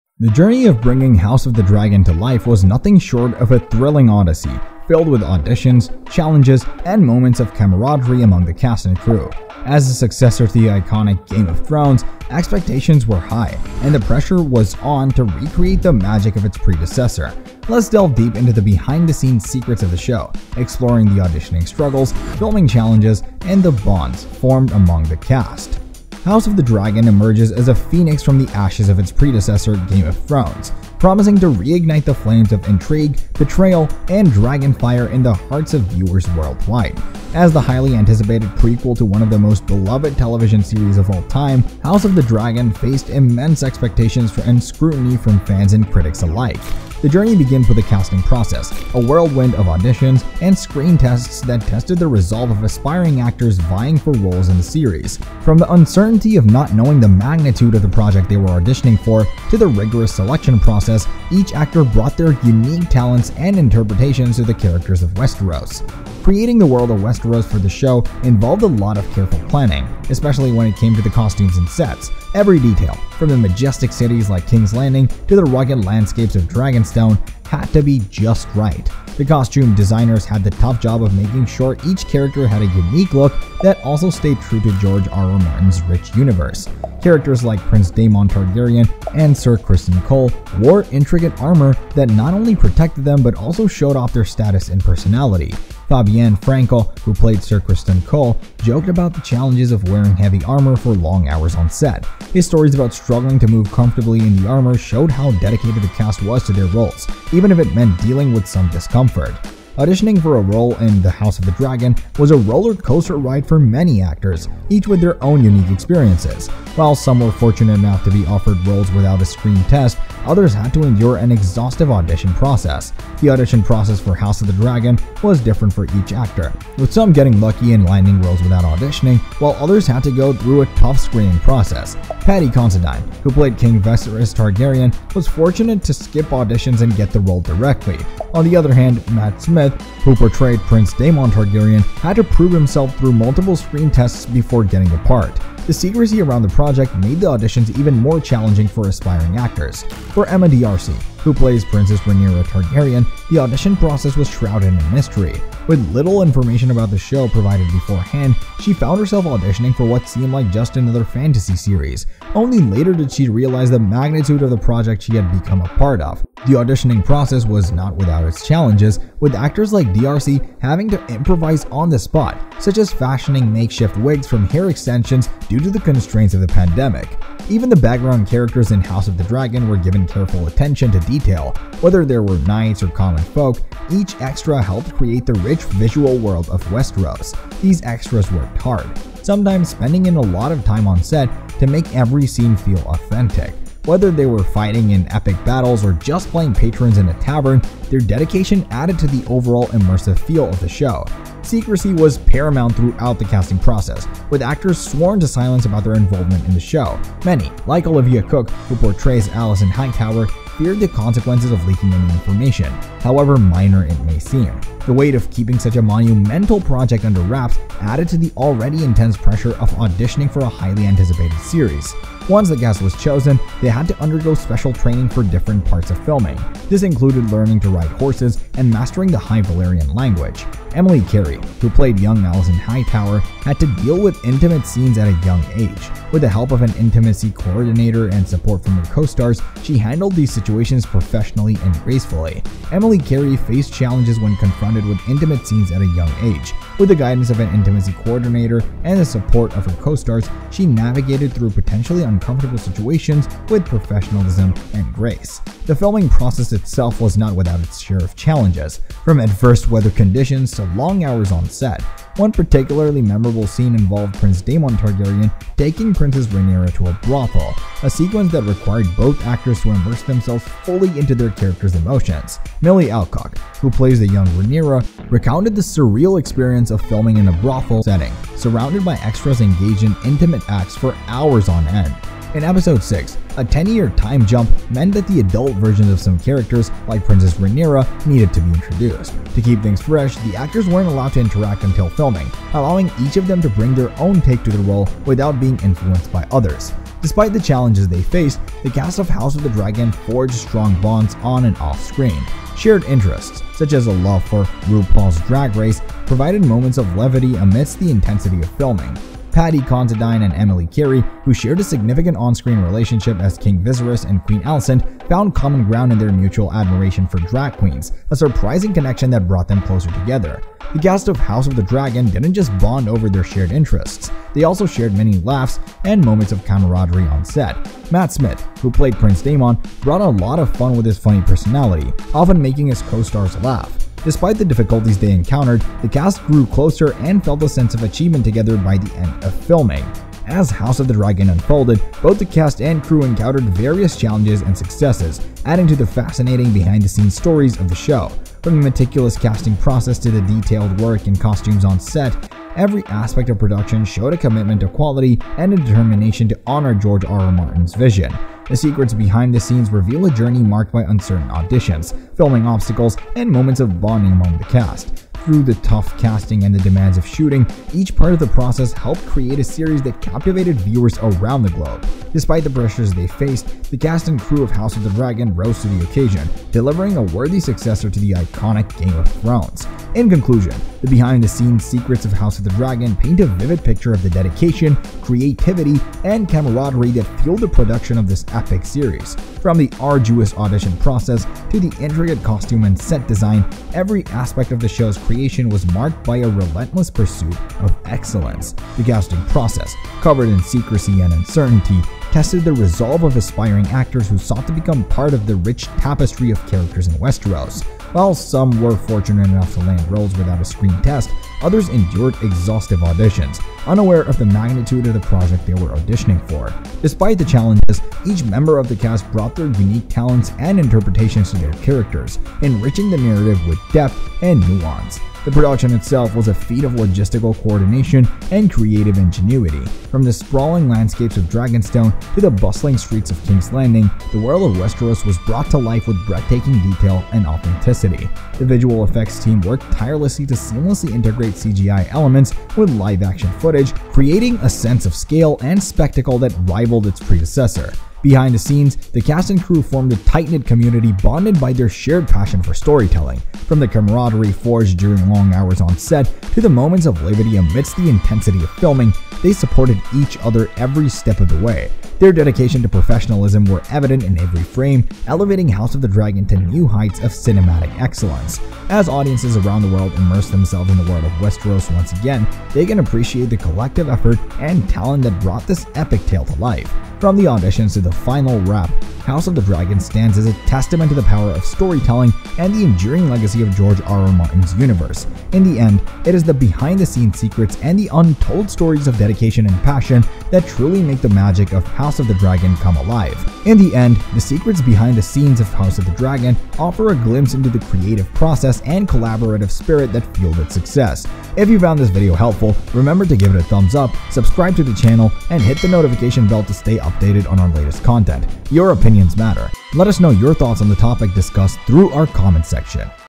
House Of The Dragon: Best Behind the Scenes On SET You NEED To Watch!
Welcome back to PhantomTV, today on the channel we're going to be talking about Behind the Scenes Secrets of House of the Dragon. If you're excited to learn more about this then make sure you watch the whole way through because you don't want to miss the details we have for you!